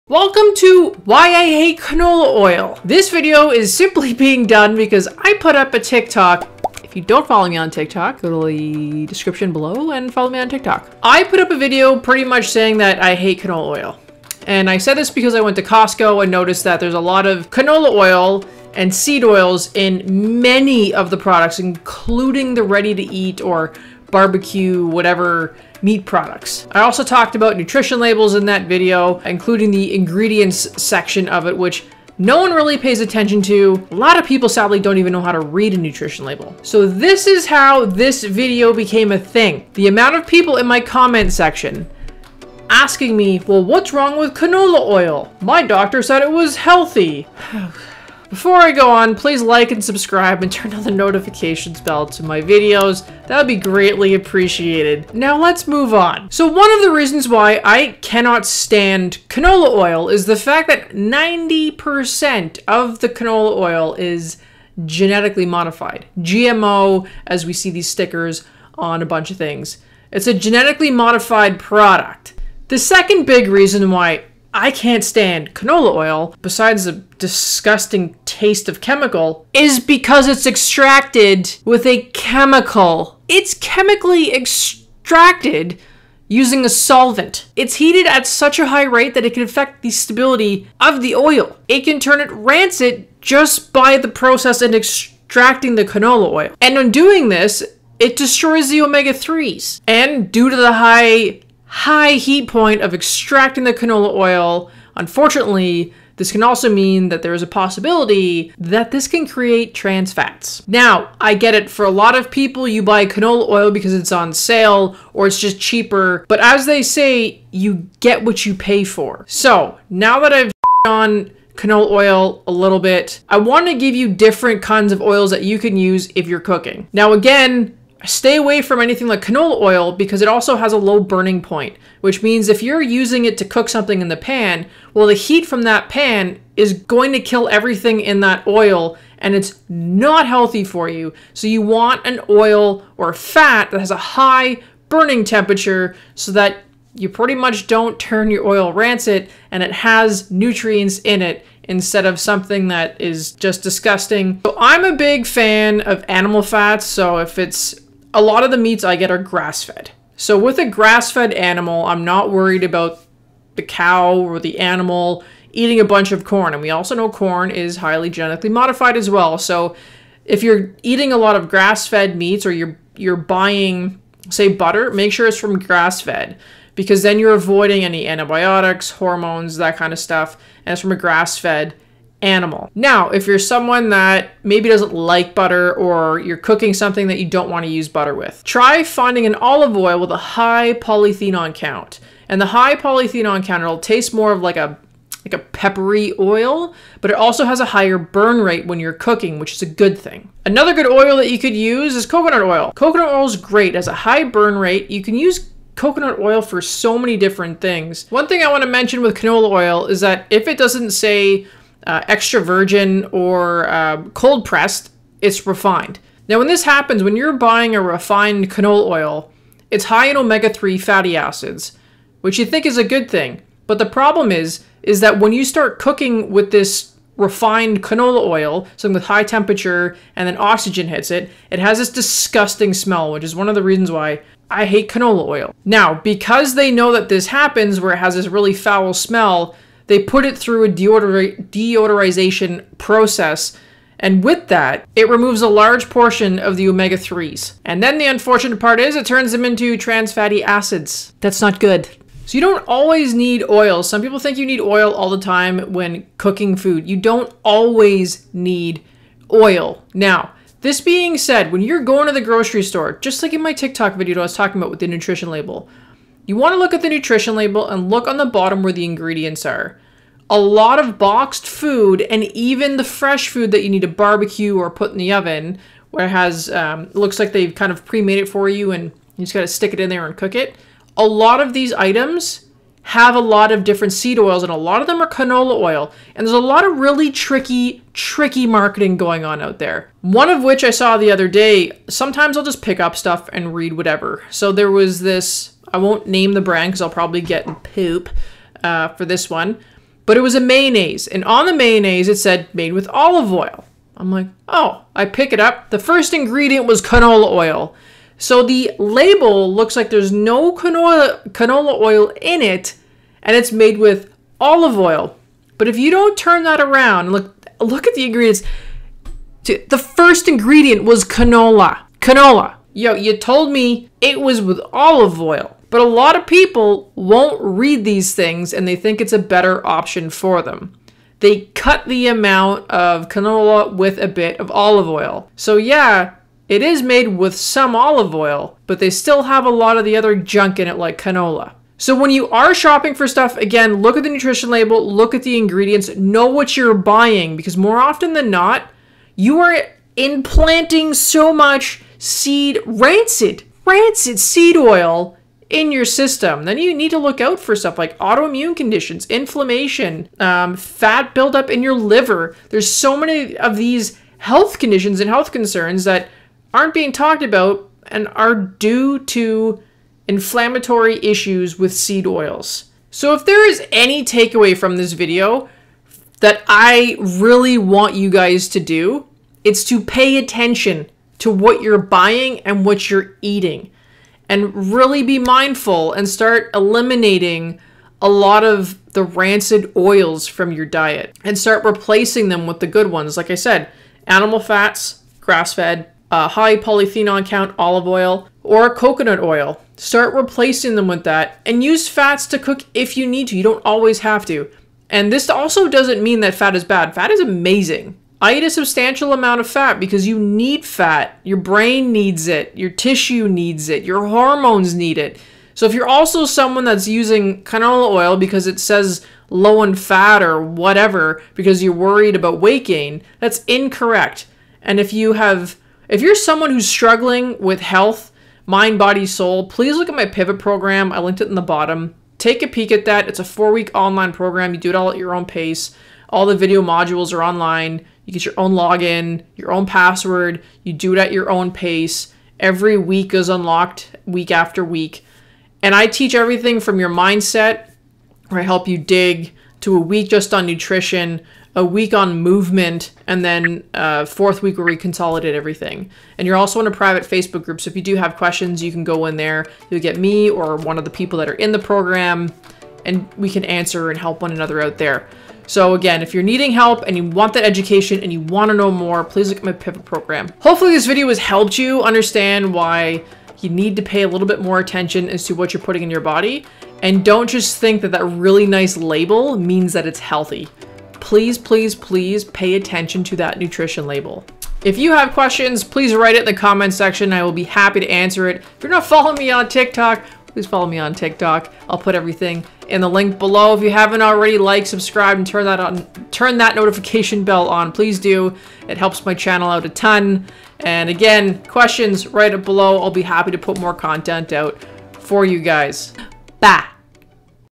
Why I HATE Canola Oil! | WHY YOU SHOULD AVOID IT!
Why are so many people using Canola Oil to cook their food?  Well, maybe it's media or the price?  Either way, we need to start educating more people on the harms of Canola oil and benefits of other fats to cook from!

In this video I speak about the process and other forms of fats you can use to cook with!